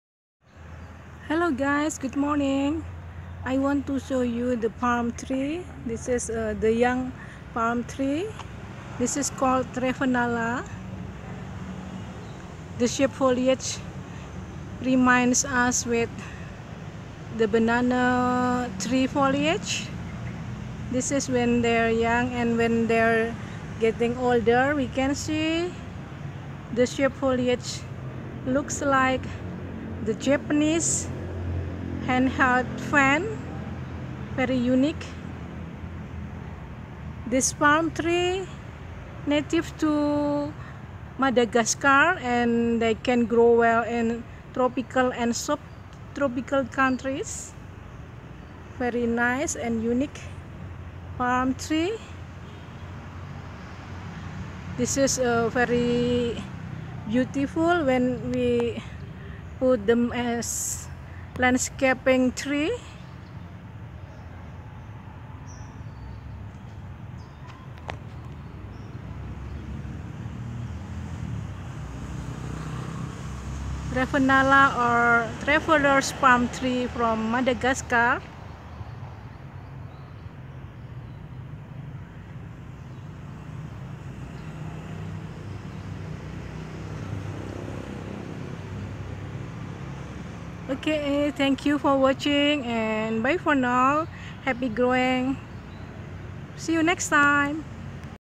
Ravenala madagascariensis Traveler's Palm Tree
Ravenala is a genus of flowering plants with a single species, Ravenala madagascariensis, commonly known as traveller's tree or traveller's palm, from Madagascar. It is not a true palm (family Arecaceae) but a member of a monocotyledonous flowering plant family, Strelitziaceae. The genus is closely related to the southern African genus Strelitzia and the South American genus Phenakospermum. Some older classifications include these genera in the banana family (Musaceae).